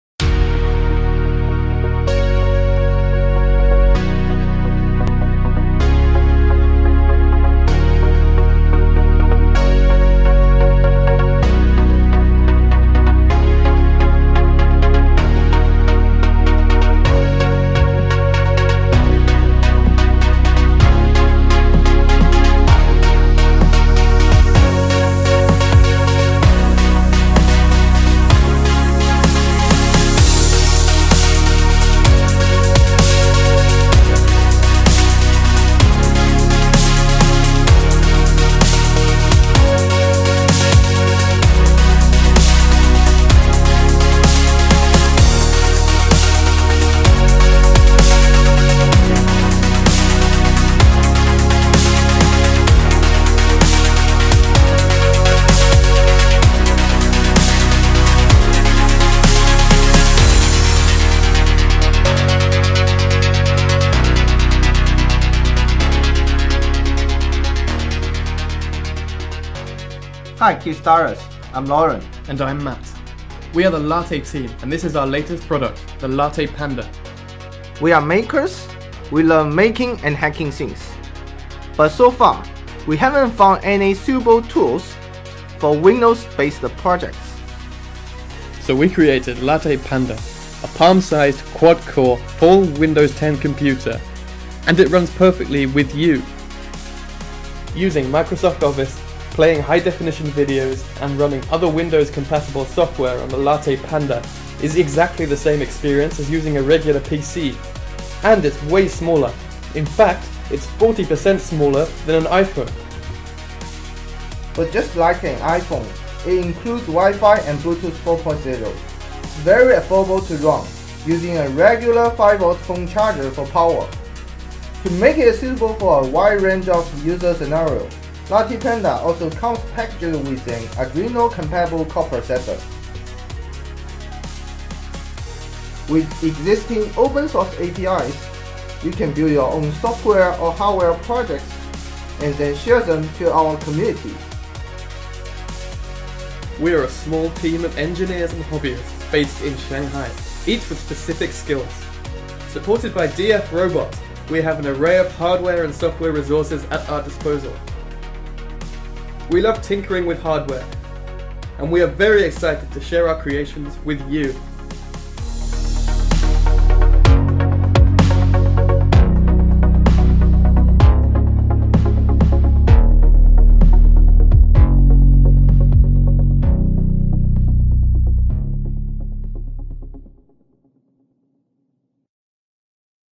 [MLAB - LattePanda] Giới thiệu LattePanda [Official Video from LattePanda]
LattePanda là một board mạch nhỏ - gọn (8.8x7.0 cm) có thể cài đặt và sử dụng Window 10 như một PC thông thường, đây là board mạch SBC đầu tiên trên thế giới  cài đặt được Windows 10.